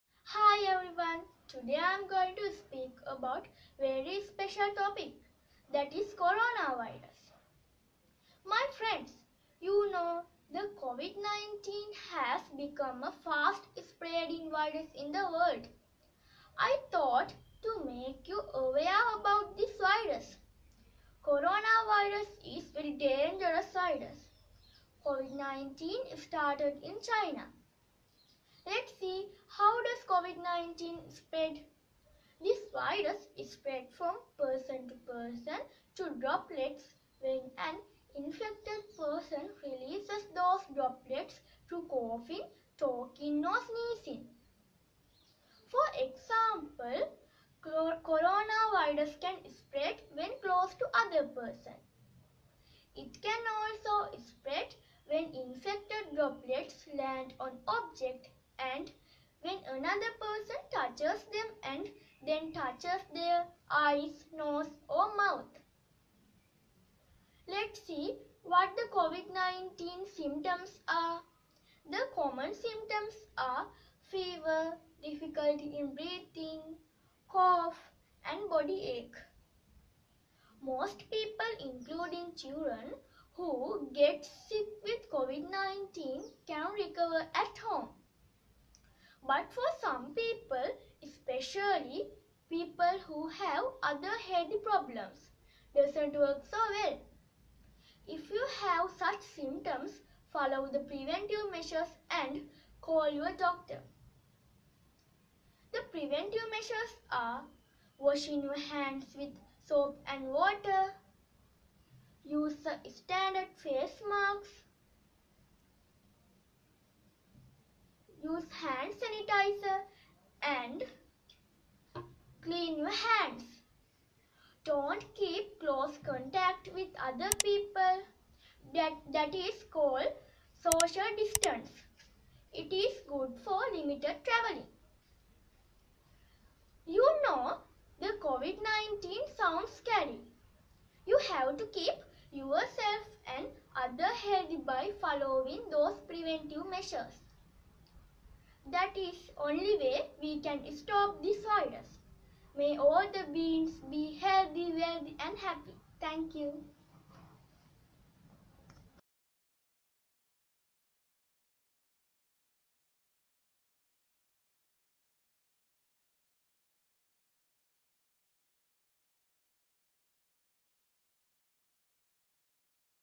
Covid 19 / Corona pandemic
Spreading, Symptoms , Preventive measures